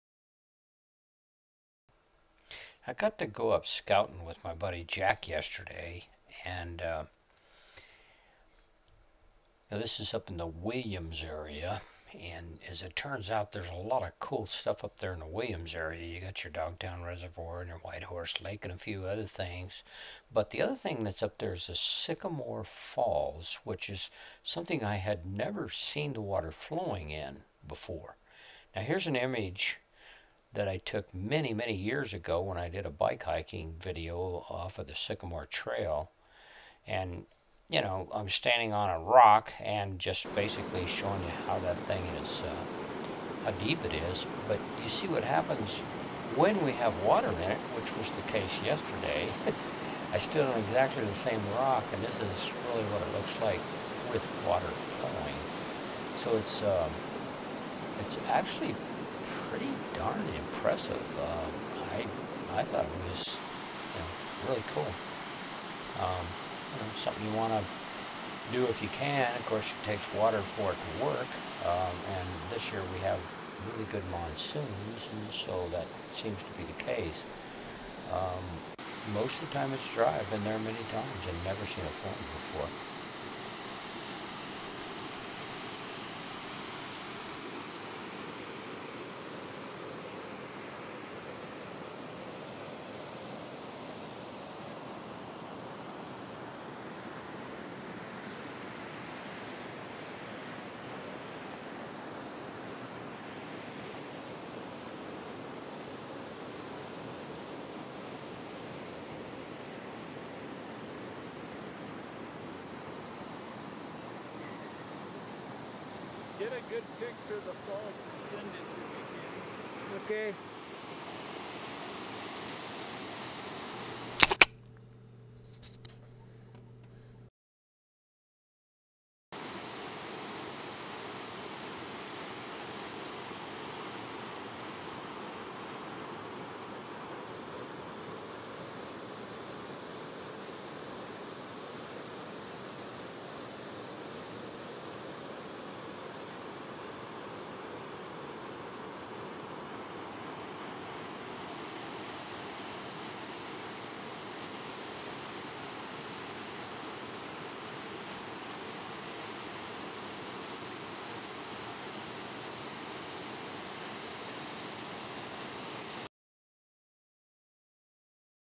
Sycamore Falls - Arizona
I have seen Sycamore falls many times before but never with the water flowing. It's really cool how the outside world changes it's personality with the conditions.

The whole area is pretty cool. If you are just passing through on I-40, the camping is great with both Dog Town and White Horse boosting established campgrounds. However, if you are like me and prefer to be away from people, the whole area is pretty much 'camp anywhere' national forest.

A word of caution, the road from Williams to Perkinsville is mostly paved while the others are good dirt roads. The monsoons can kick up in a heartbeat so be very mindful of flowing water across the roads.

Have a look at our websites for various other camp ideas and tips: